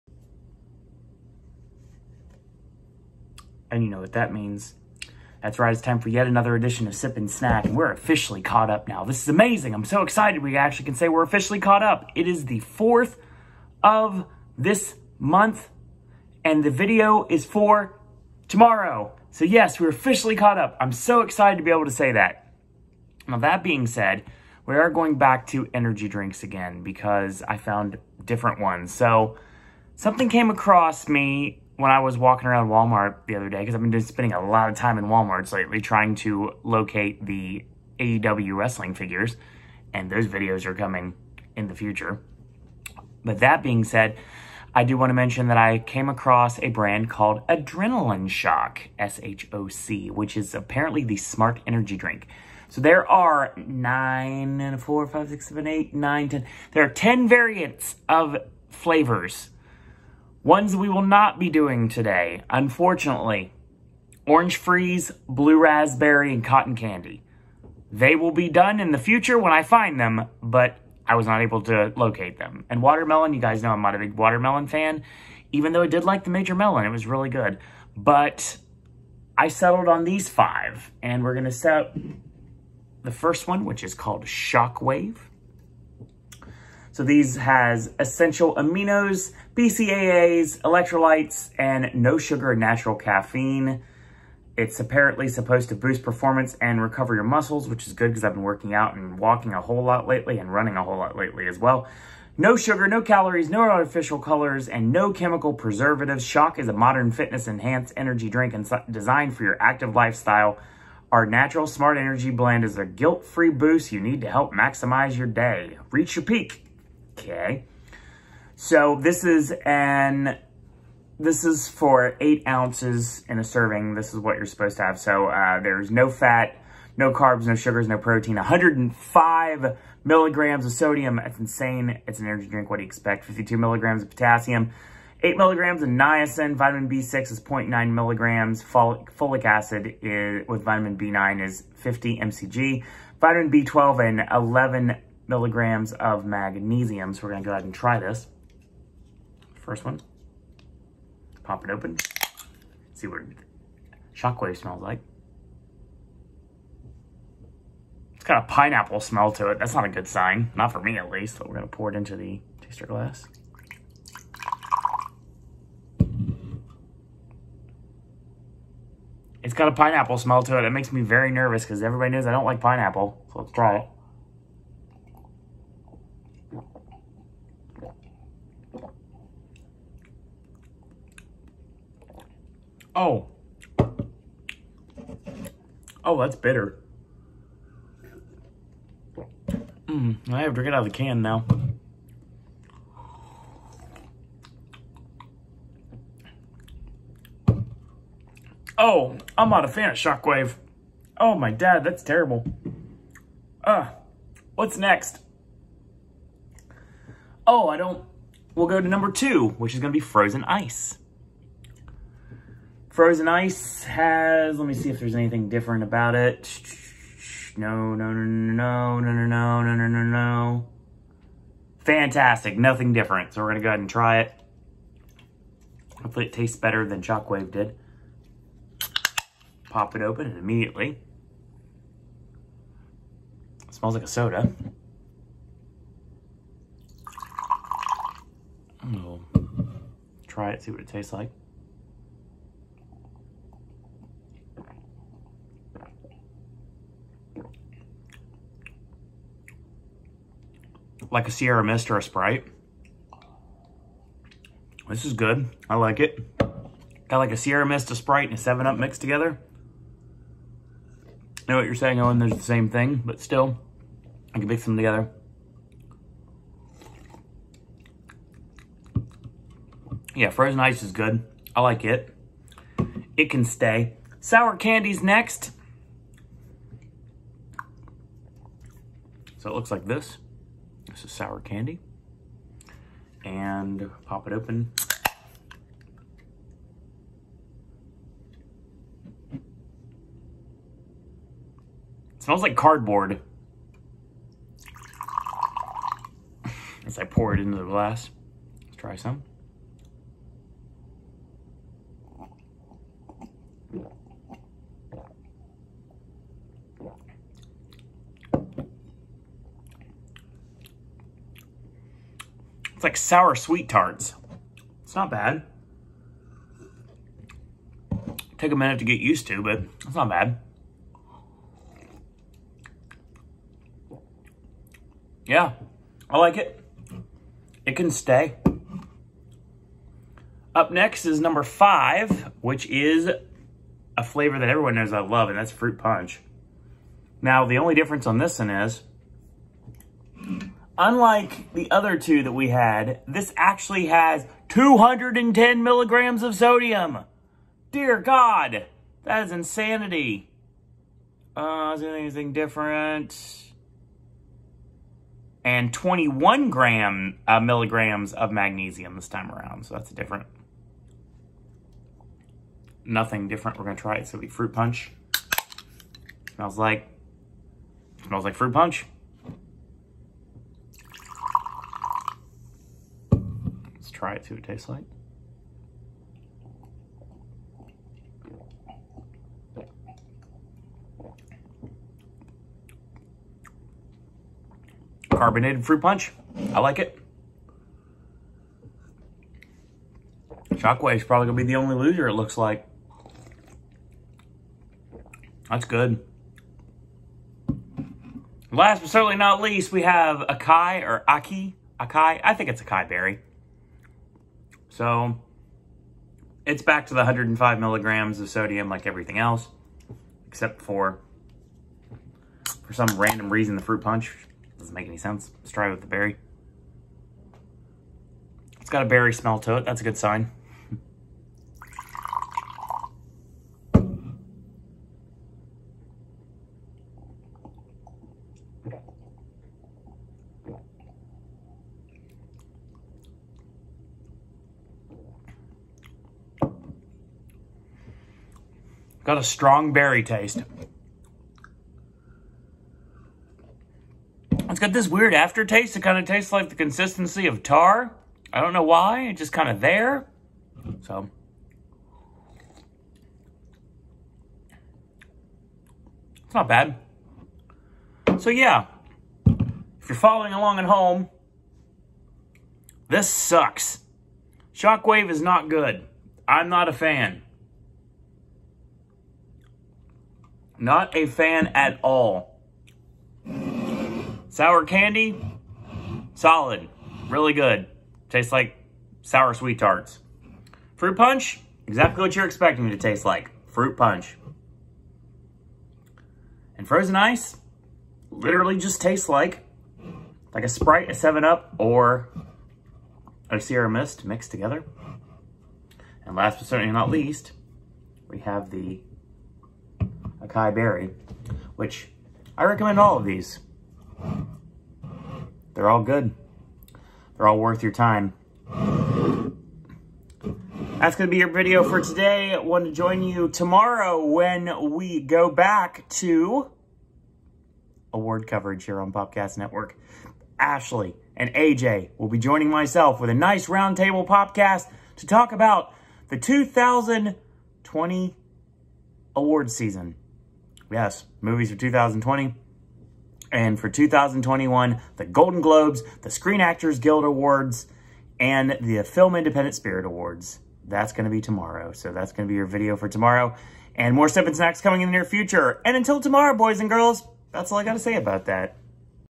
Sip & Snack: Adrenaline Shoc Energy Drinks(2-5-21)
Today I was able to try out another new energy drink on the market, Adrenaline Shoc. Today I try out 5 of the 10 flavors in Shoc Wave, Frozen Ice, Sour Candy, Fruit Punch & Açaí Berry.